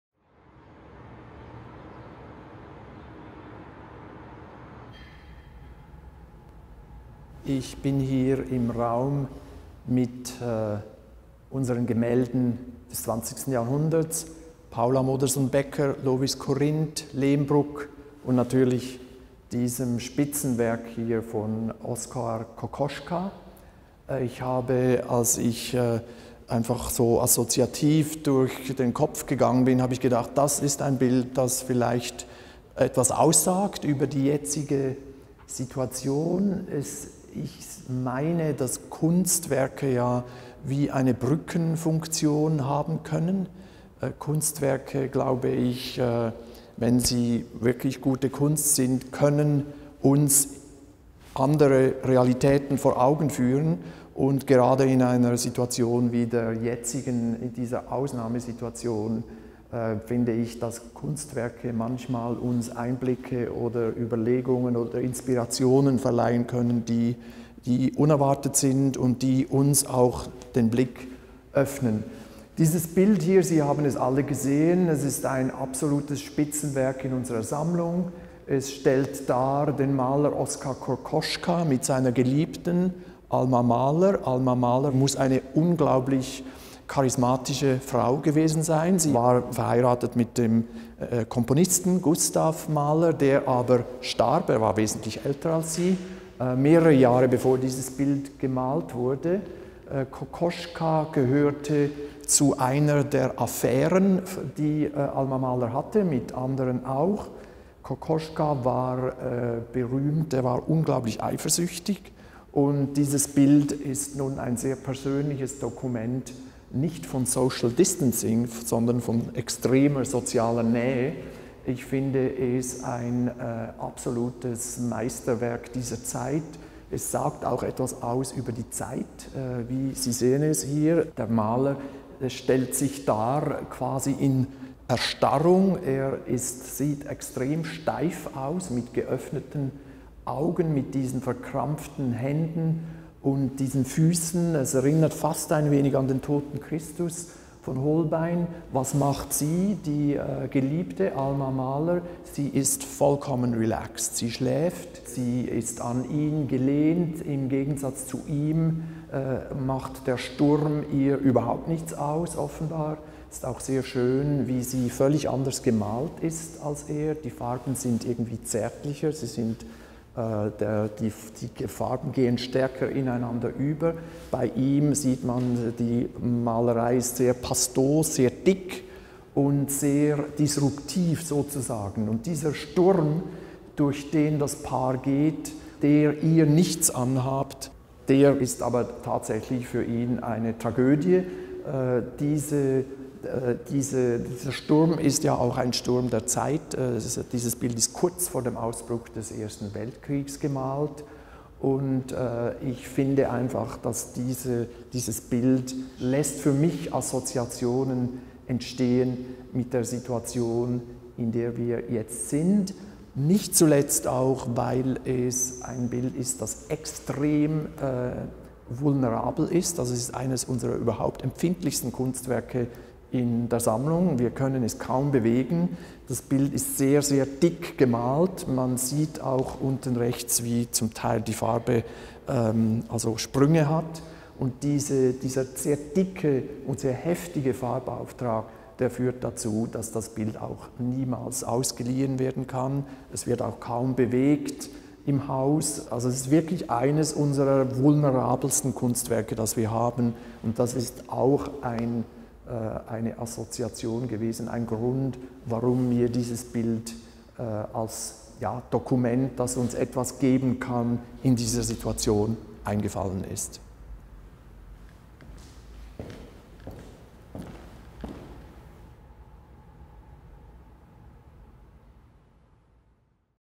Oskar Kokoschka, Die Windsbraut, 1913
Josef Helfenstein (Direktor) über das Werk "Die Windsbraut" des Künstler Oskar Kokoschka, über seine Relevanz in der Ausnahmesituation durch COVID-19.

Credit: Die Windsbraut, "Die Windsbraut", 1913, Öl auf Leinwand, 180.4 x 220.2 cm, Kunstmuseum Basel, mit einem Sonderkredit der Basler Regierung erworben 1939

Weitere Werke des Künstlers in der Sammlung des Kunstmuseums Basel finden Sie